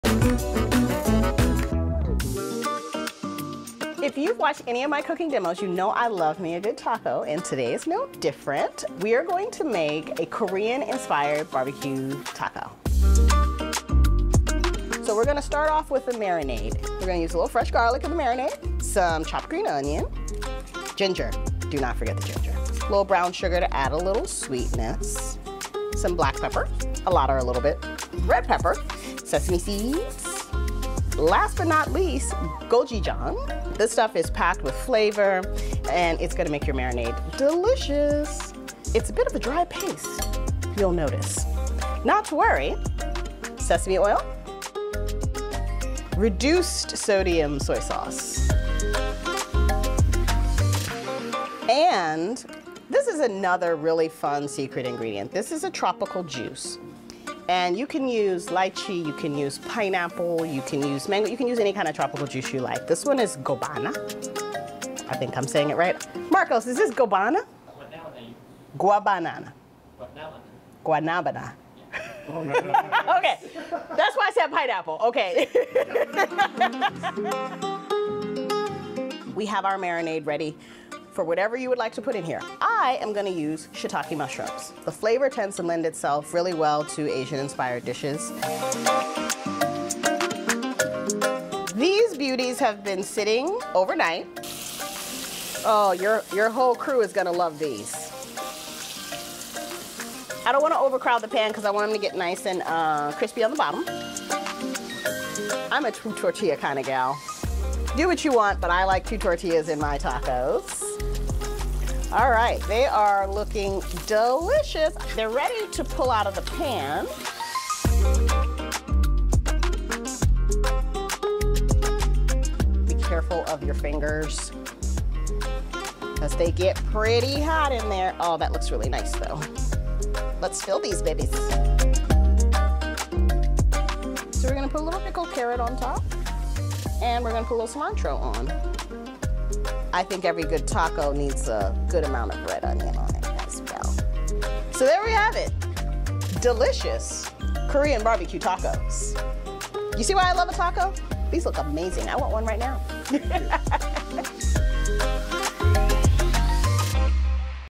A Korean-inspired barbecue taco - Makini's Kitchen
Ginger, brown sugar, gochujang and tropical juice are just a few ingredients that give the marinade its kick.